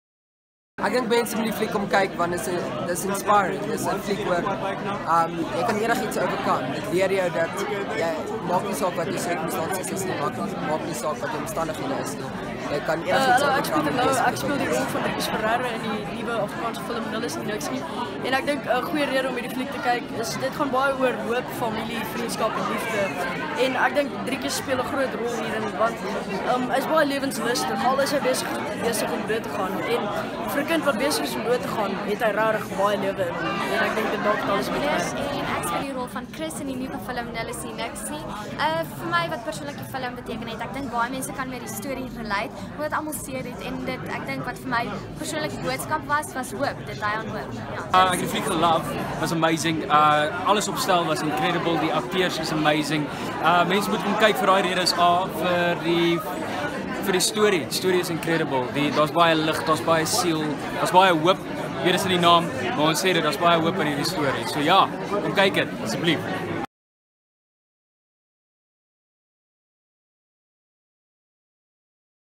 Nul Is Nie Niks Nie
Stage and Screen het met die akteurs van Nul is nie Niks nie gaan gesels by die Premiere